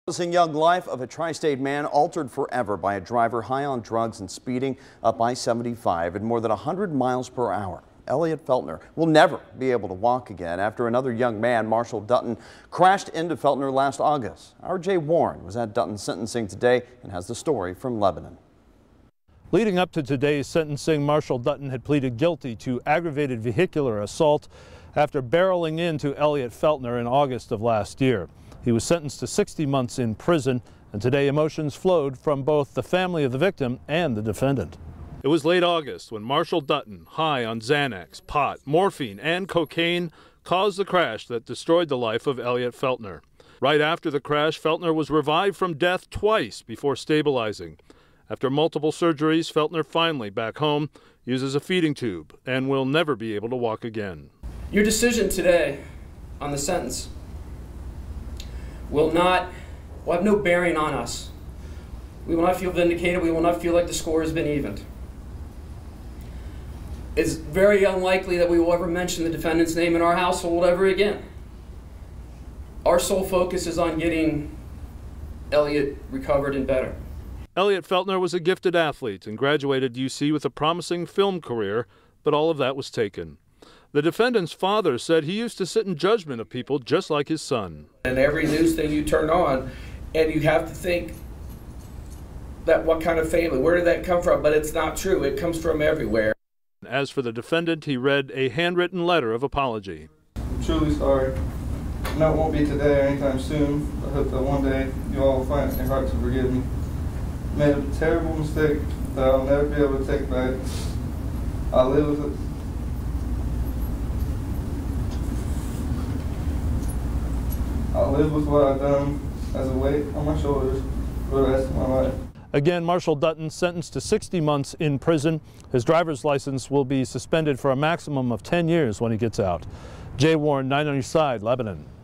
Drugged driver sentenced to five years in jail
Marshall Dutton pleaded guilty to aggravated vehicular assault after barreling into Elliot Feltner in August 2016. Dutton was sentenced to 60 months in prison; Feltner will never walk or eat on his own again.